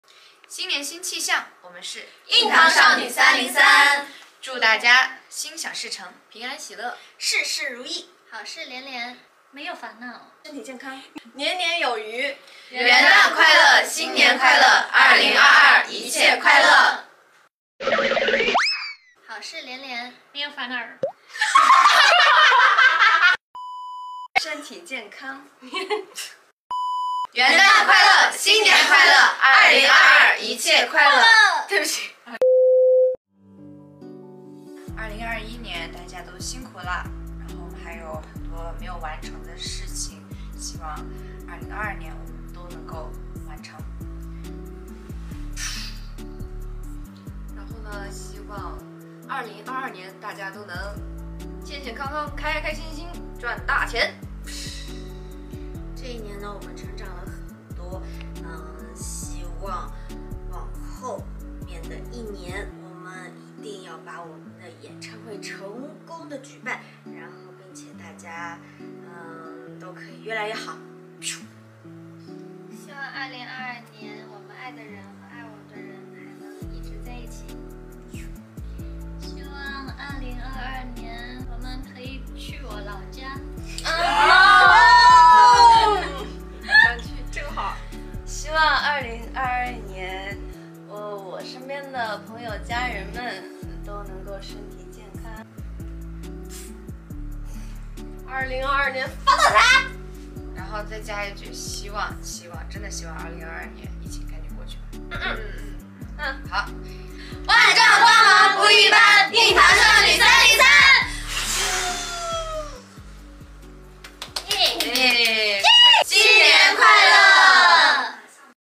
[ENG SUB] 硬糖少女303 新年祝福+新年愿望 BonBonGirls 303 New Year Wishes | 陈卓璇 Chen Zhuoxuan 20220101
新年起始，元气满满开启崭新旅程🎁
元旦佳节，喜气洋洋迎接新年到来🎉
新年祝福送达～新的一年愿糖纸们健康顺遂、快乐相伴[给你小心心]2022也请陪伴女孩们一起gogogo

一岁一礼，一寸欢喜❤️
少女满怀憧憬，借“蜡烛”许愿2022万事顺遂～
点击视频，收获更多糖妹们的甜蜜祝福吧㊗️万丈光芒不一般，硬糖少女303！